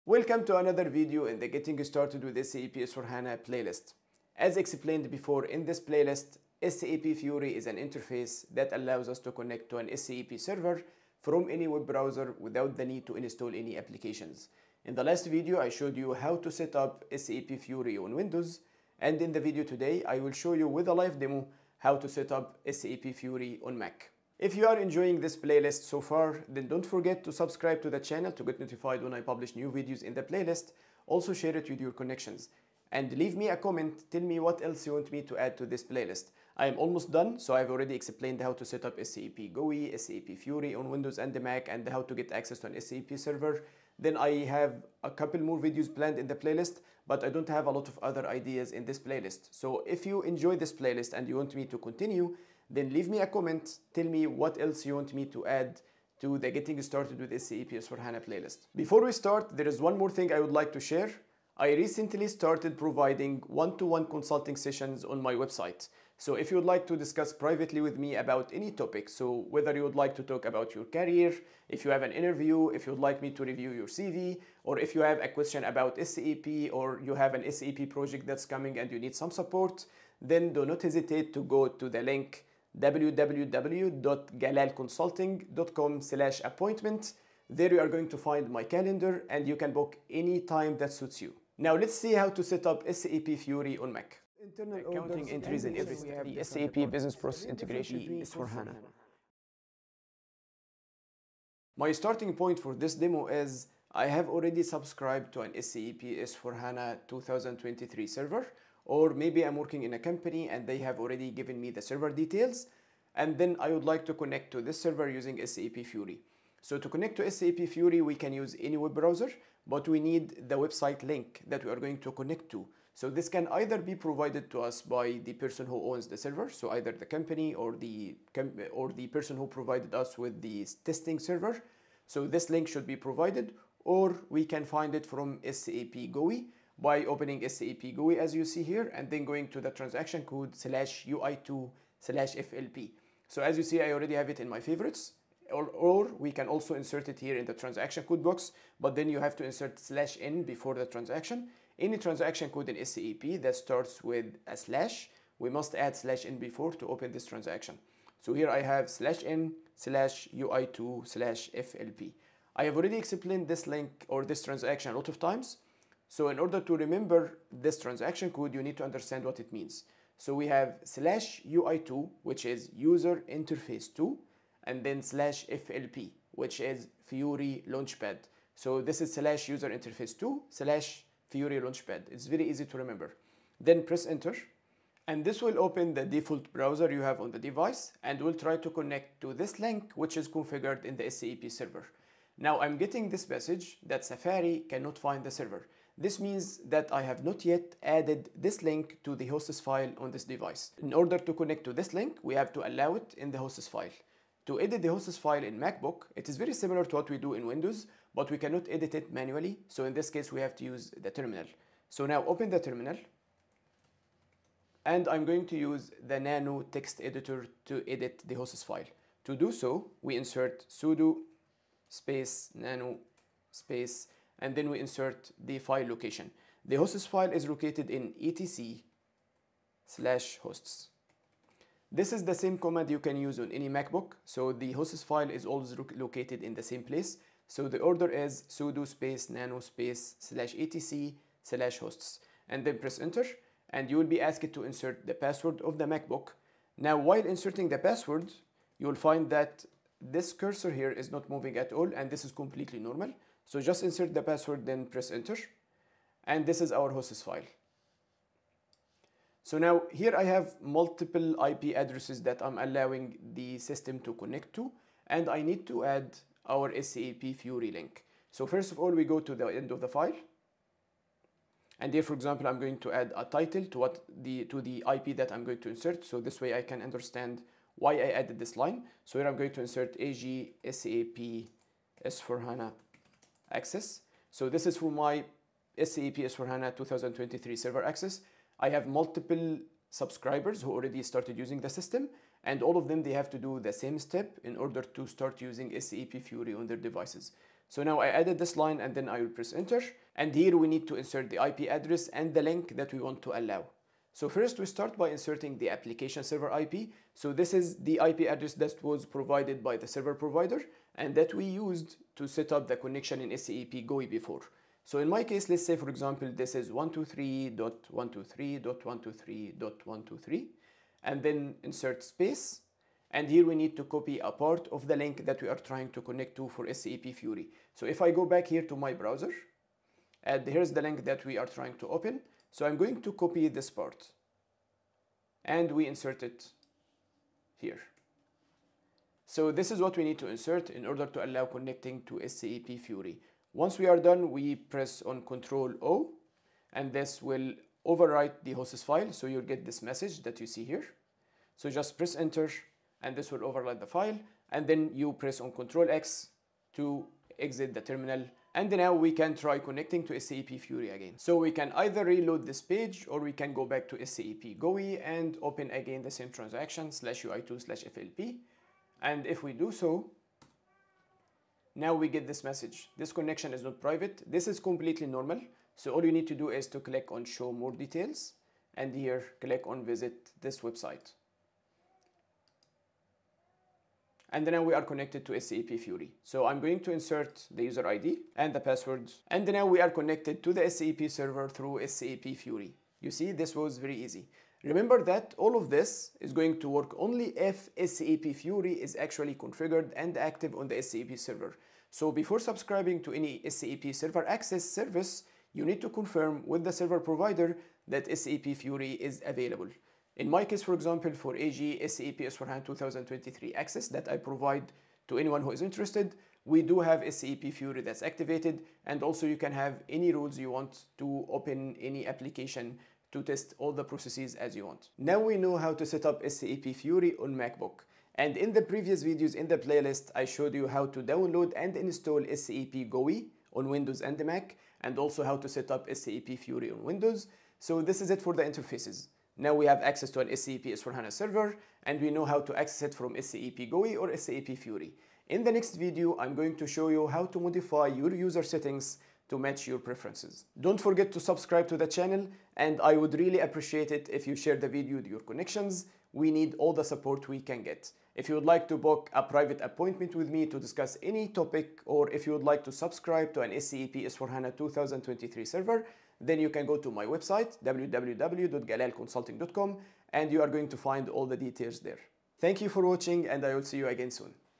Enable SAP FIORI on Mac: Update Hosts File
WelcomToday, I’ll guide you step-by-step on how to set up SAP FIORI on your Mac by updating the hosts file. This demo will help you connect to SAP FIORI using any web browser without installing additional apps.